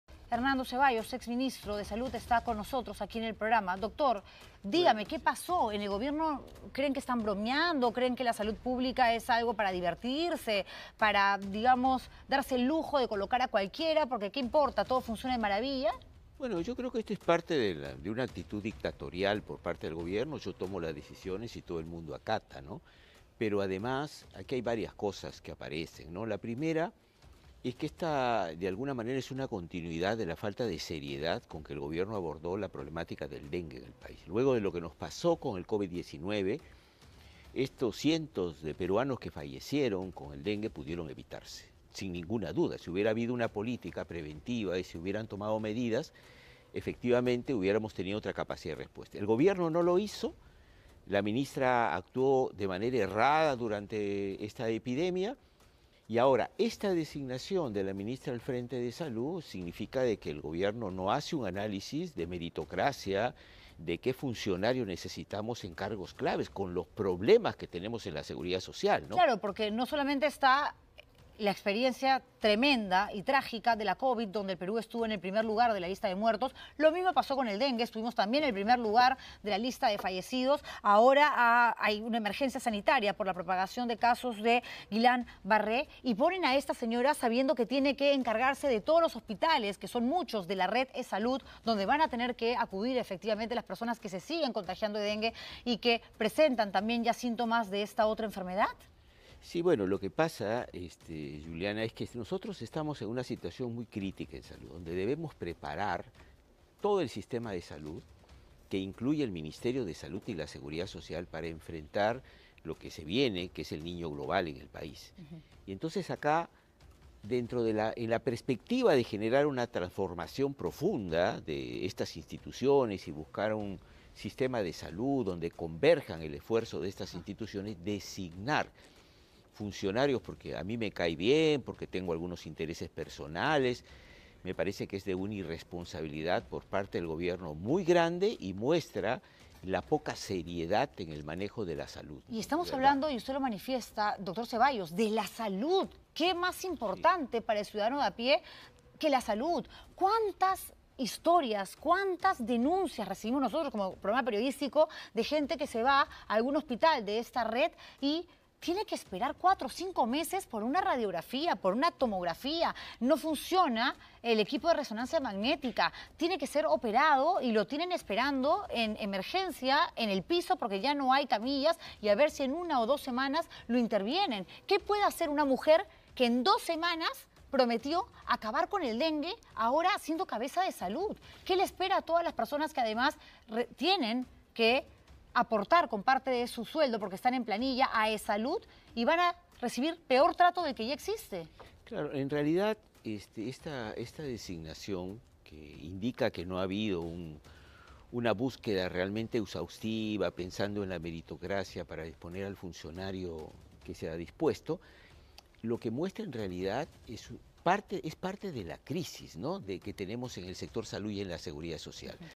Hernando Cevallos por designación de Rosa Gutiérrez: "Es una actitud dictatorial del Gobierno"
El exjerarca señaló que también es parte de una continuidad de la falta de seriedad con la que se abordó la problemática del dengue.